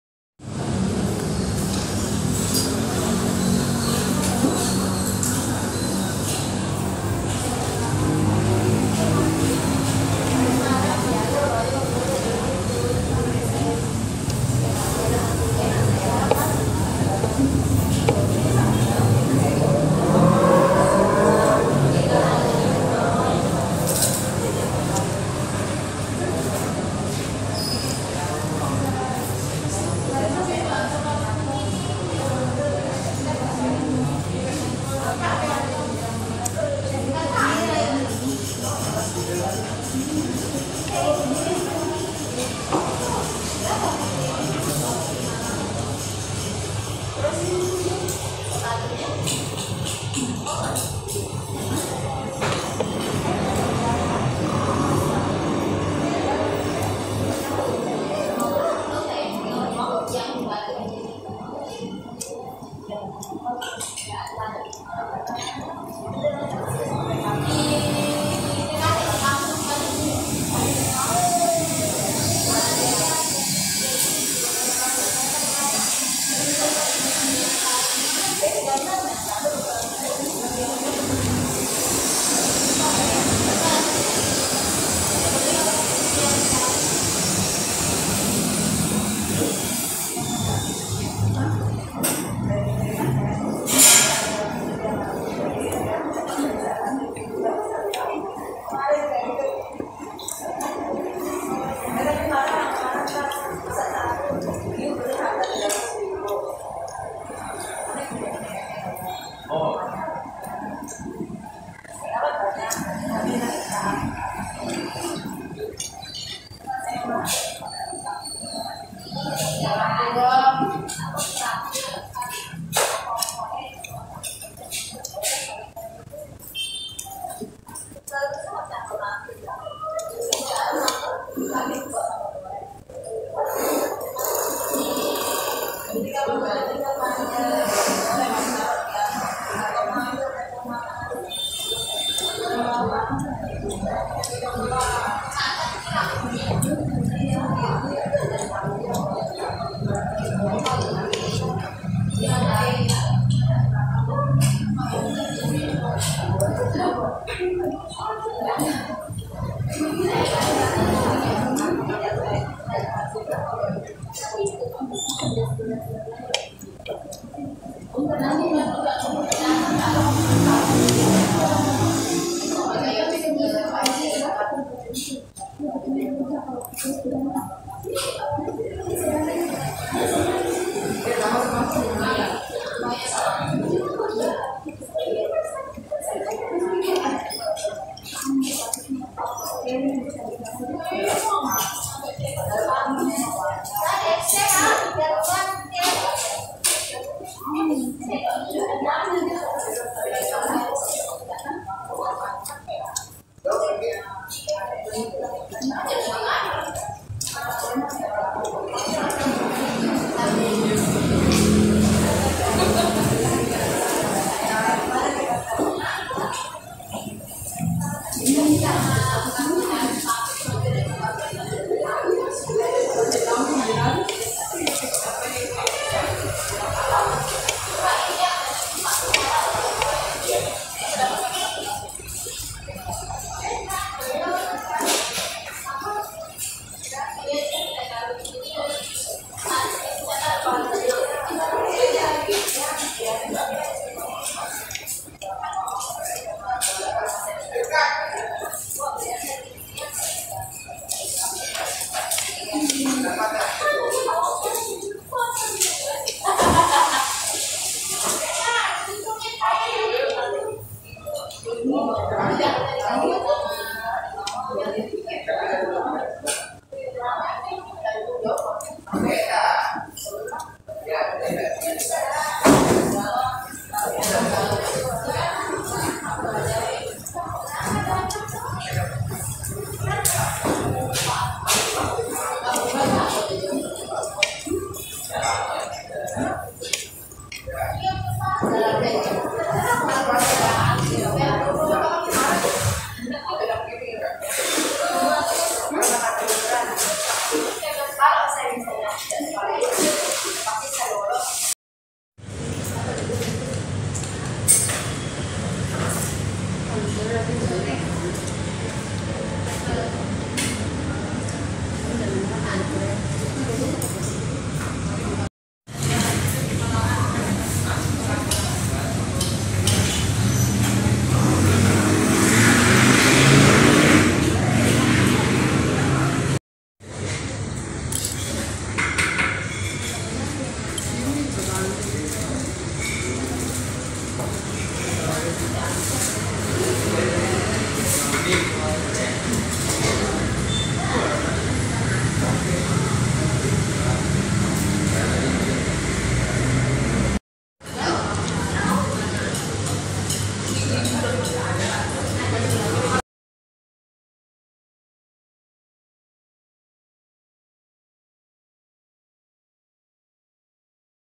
Fried Rice Indonesia Food and Jmie Pedesss Dahsyat
Fried Rice is Deliciouse Food from Indonesia

Located Restaurant JMie pedesss Dahsyat; Jln. Transmigrasi No 2 Monjok Timur. Mataram Lombok.

Rarey Pujha Channel